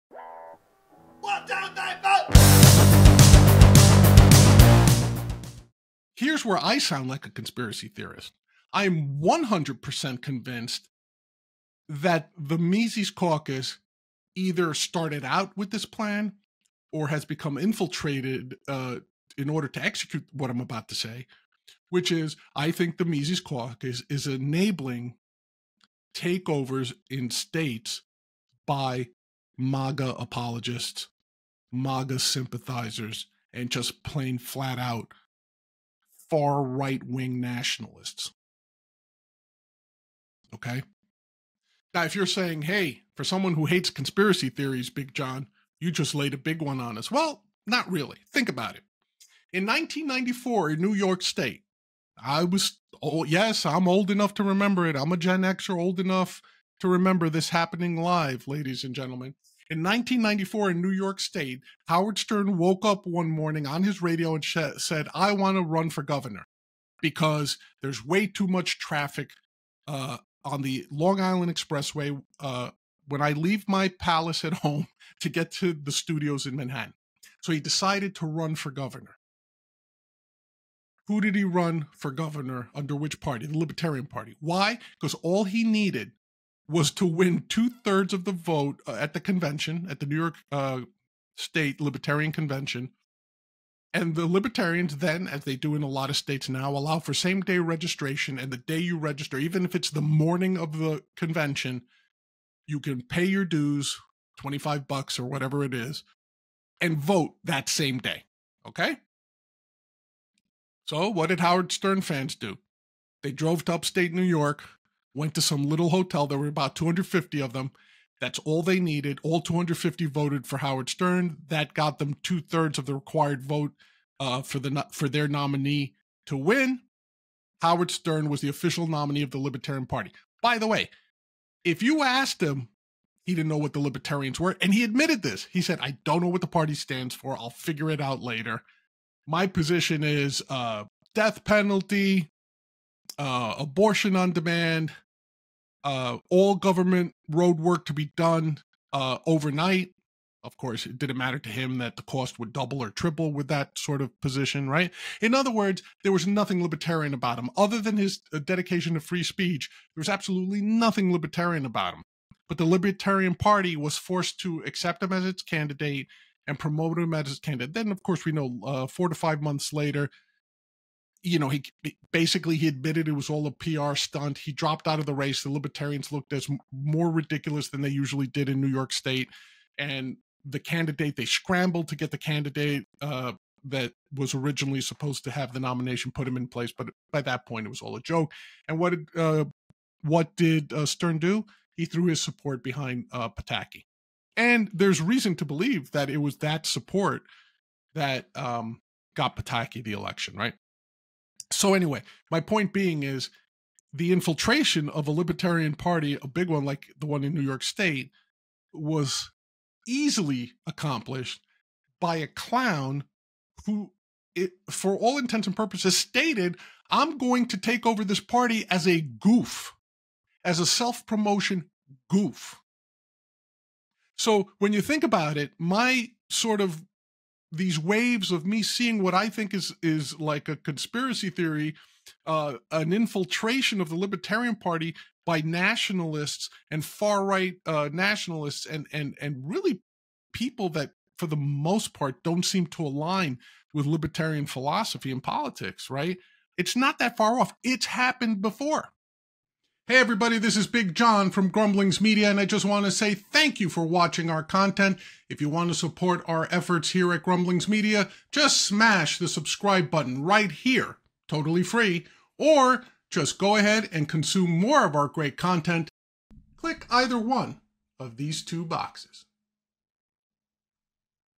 Libertarian Party Crisis: Examining the Mises Caucus - A MAGA Conspiracy?| @Grumblings Media
In this video, we delve into the ongoing crisis within the Libertarian Party and explore the controversial role of the Mises Caucus. Is it a genuine libertarian movement or is there a connection to MAGA (Make America Great Again)? 🤔 Join us in this investigative journey to uncover the truth!

🔍 Timestamps:
00:00 Opening
00:30 Is the Mises Caucus a MAGA plot?
01:00 How Howard Stern took over the NY Libertarian Party in 1994

📌 Key Points:
❶ The Mises Caucus: Understand the origins, goals, and influence of the Mises Caucus within the Libertarian Party.
❷ MAGA Connection?: Investigate claims and conspiracy theories suggesting a potential link between the Mises Caucus and the MAGA movement. Separating fact from fiction!
❸ Party Dynamics: Explore how the presence of the Mises Caucus has impacted internal dynamics, elections, and policy discussions within the Libertarian Party.
❹Interviews and Insights: Hear from party members, analysts, and experts as we gather diverse perspectives on the Mises Caucus and its role in the Libertarian Party.
❺ Historical Context: Gain a historical perspective on the Libertarian Party and how recent developments might be shaping its future.
❻Impact on Libertarian Ideals: Discuss the potential impact of these developments on the party's commitment to libertarian principles.